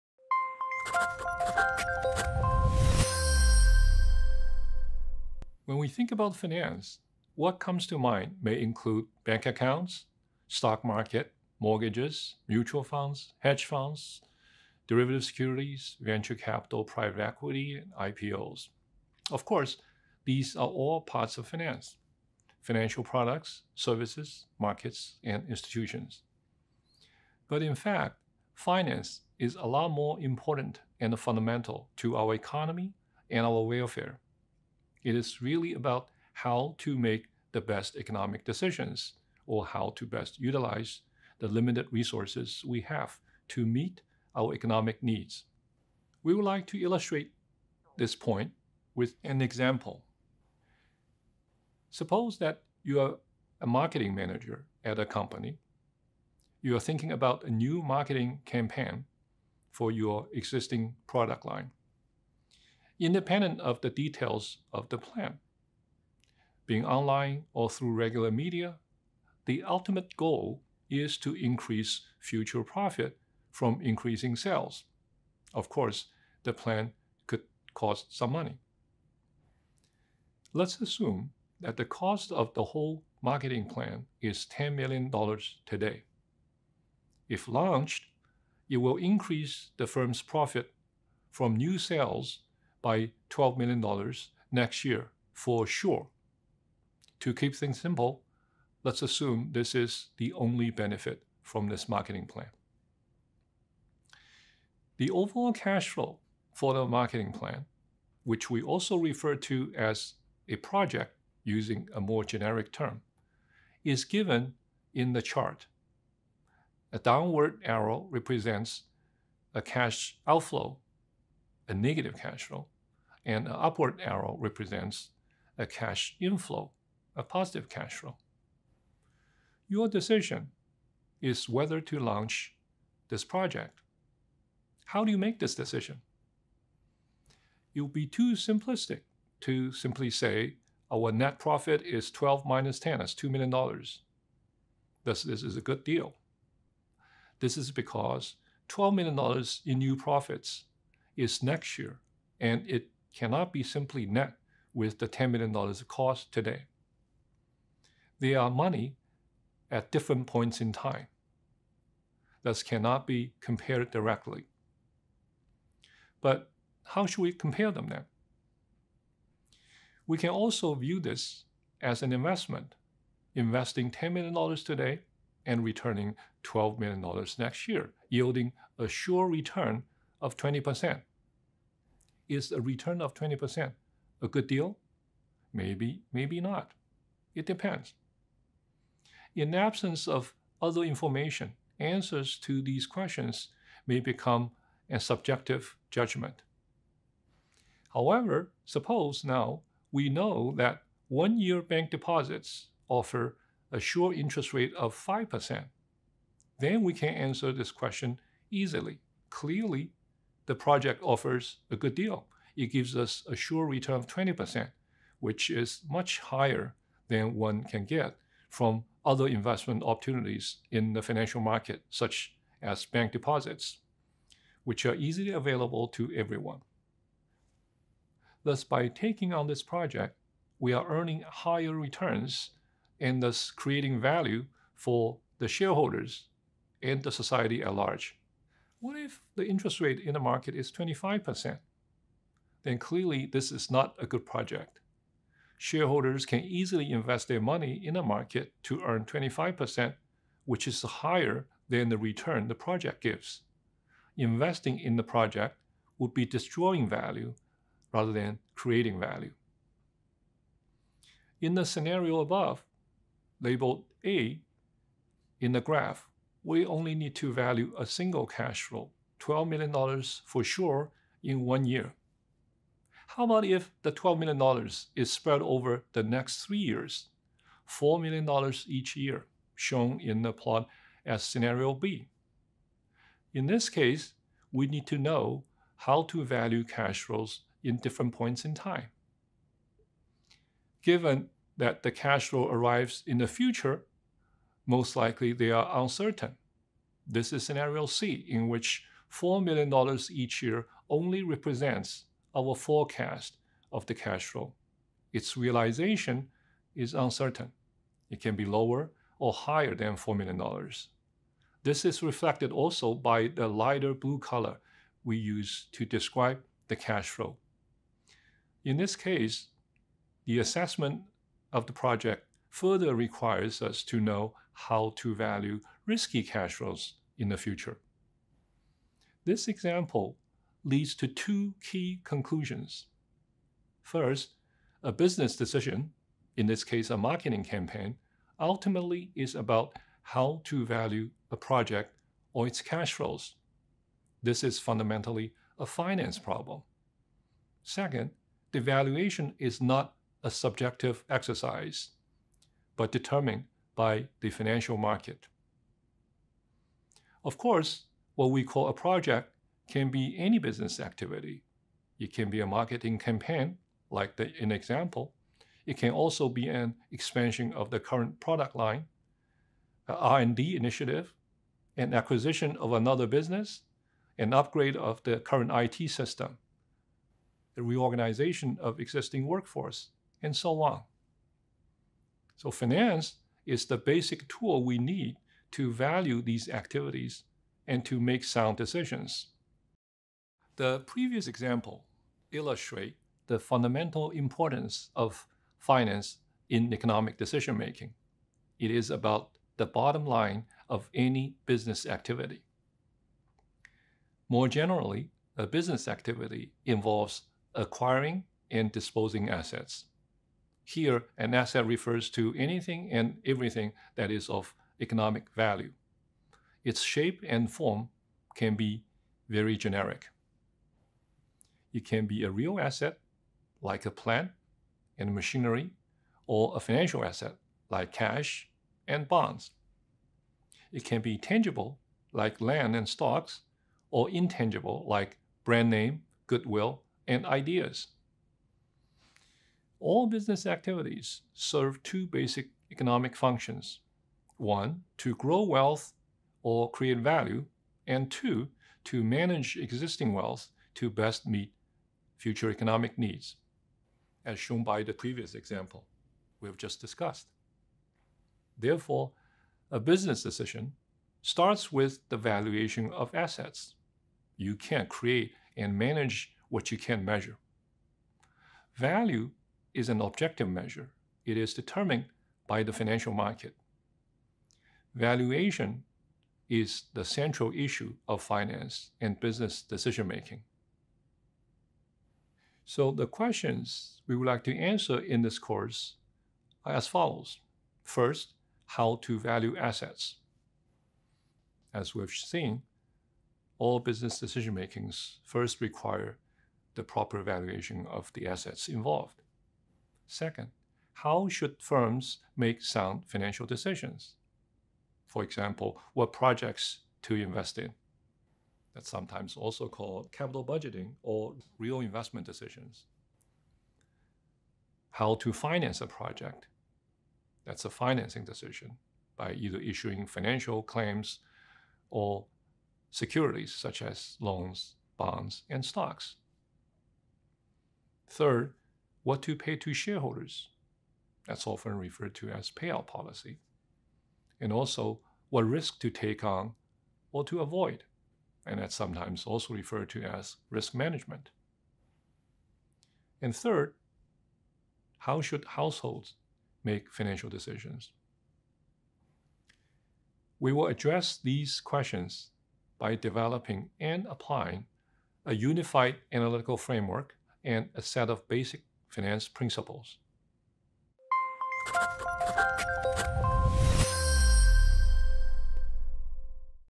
What is Finance?
Professor Jian Wang uses the example of a marketing campaign to create and grow value to illustrate why finance is a basic tool used to value projects.